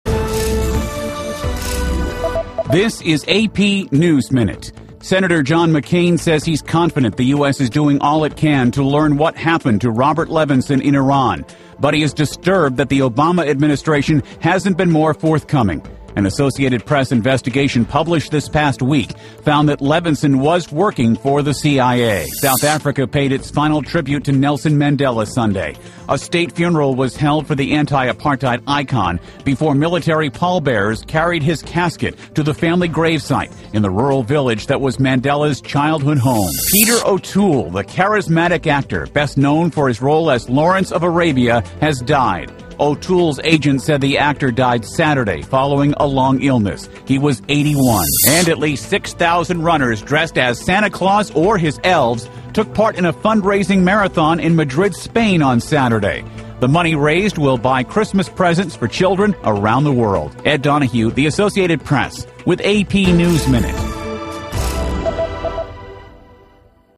AP Top Stories Dec. 15 P
Here's the latest news for Sunday, December 15th. Senator McCain on Robert Levin disappearance;  Mandela final farewell; Actor Peter O'Toole dies and Santa goes running in Spain for charity.